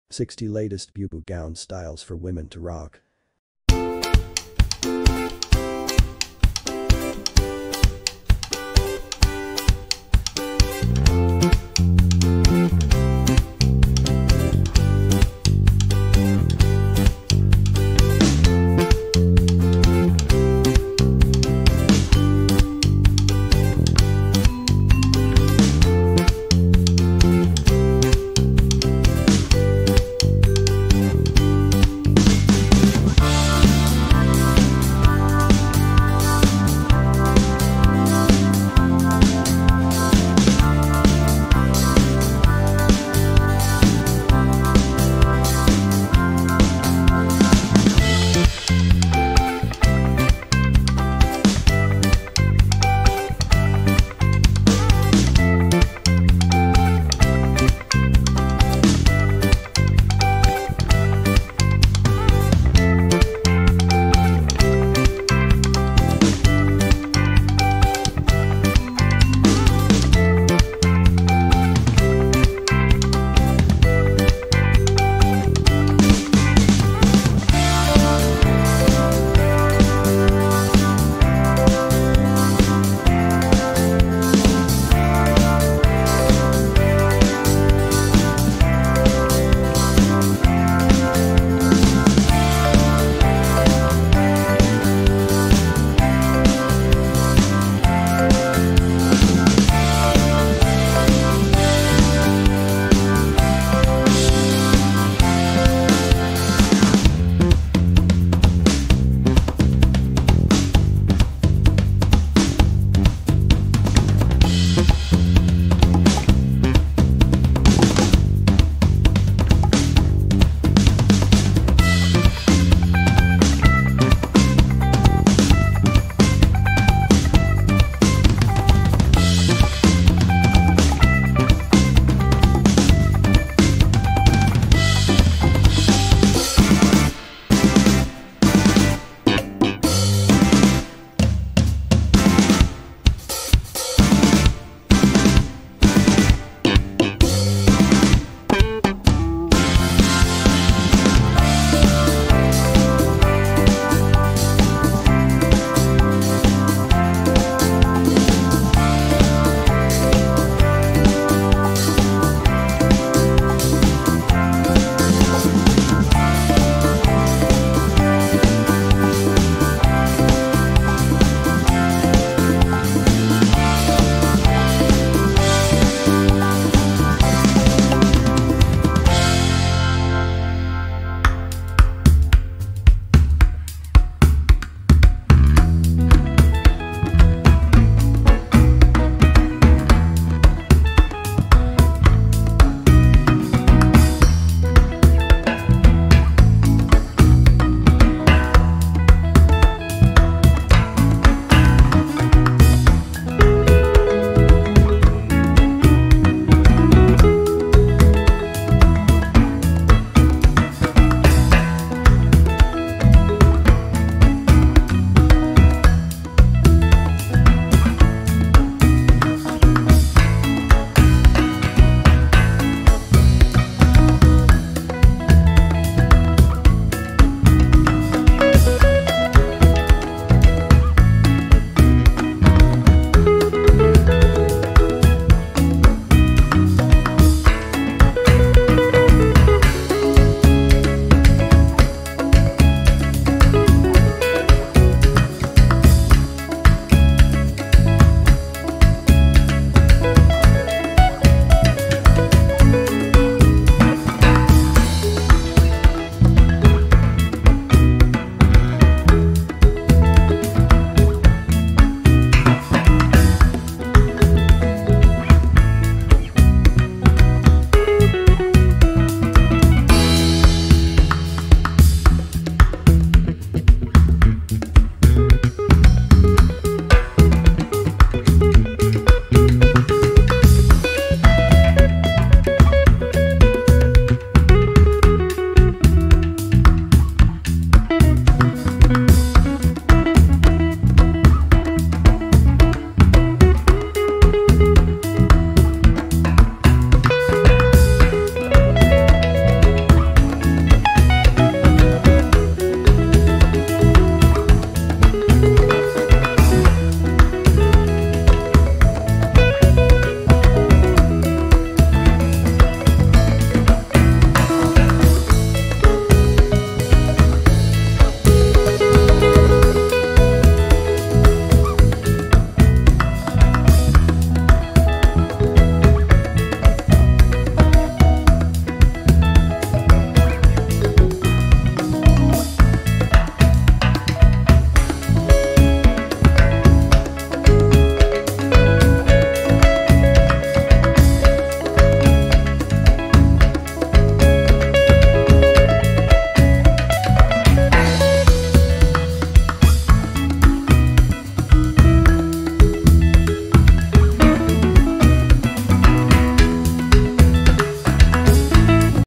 60 Bubu Gown Styles for Women (Trending Styles)
Simplicity is the rule of the game here. This unique Bubu wear will help you achieve an elegant look. Hire our services. We handle male and female wear styling and sewing. We deliver anywhere in the world.